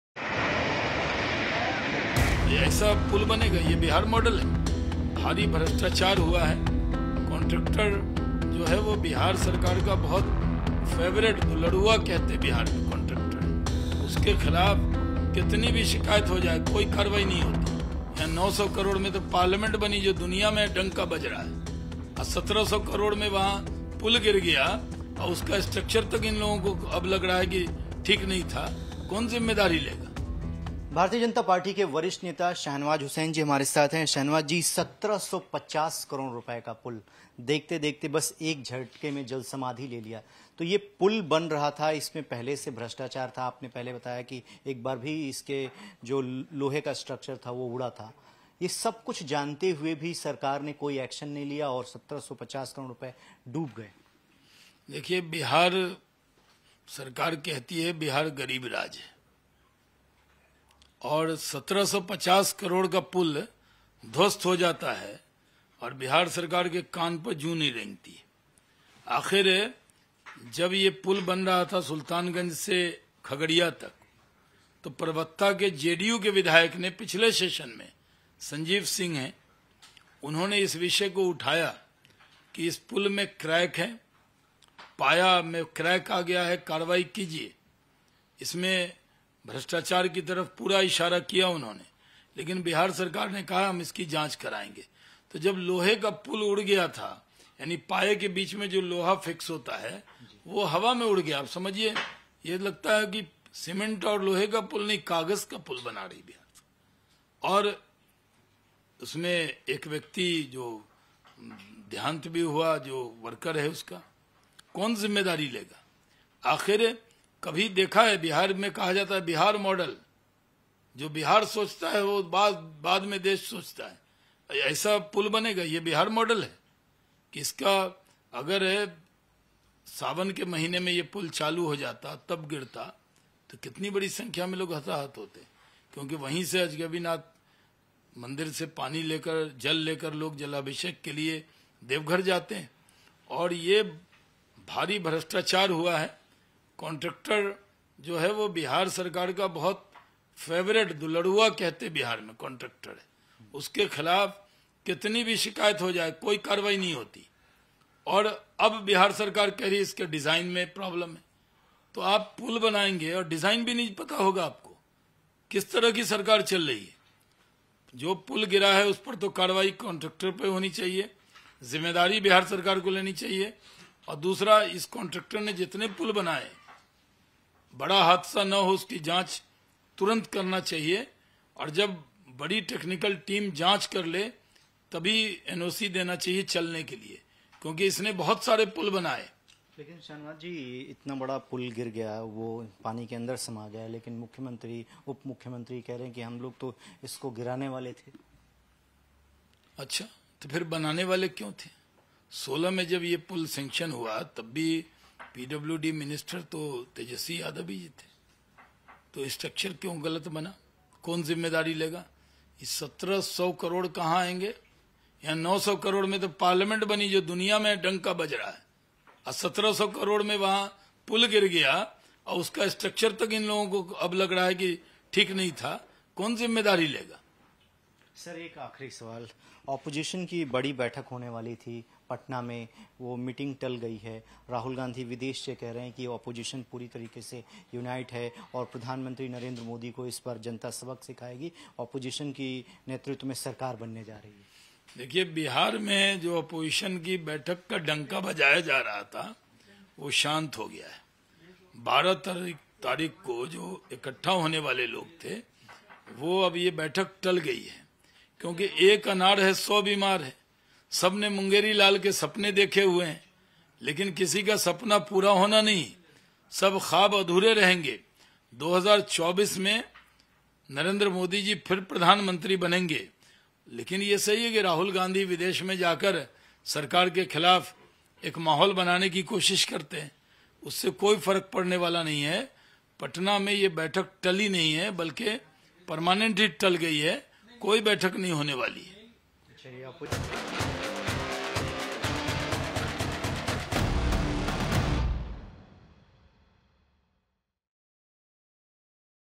Bhagalpur Bridge Collapse: BJP ने साधा Nitish Kumar पर निशाना | Hindi News
Nitish Kumar पर निशाना साधते हुए कहा कि 1700 करोड़ का पुल गिर जाता है, और 900 करोड़ का नया संसद बनता है तो पूरे देश में इसकी चर्चा होती है, क्या बिहार सरकार कागज का पुल बना रही थी, ये कांट्रेक्टर बिहार सरकार का सबसे प्रिय व्यक्ति है इसके खिलाफ, जेडीयू के नेता ने सदन में आरोप लगाया था, लेकिन इसके बाद भी कोई कारवाई नही हुई, इसलिए इसकी जांच होनी चाहिए.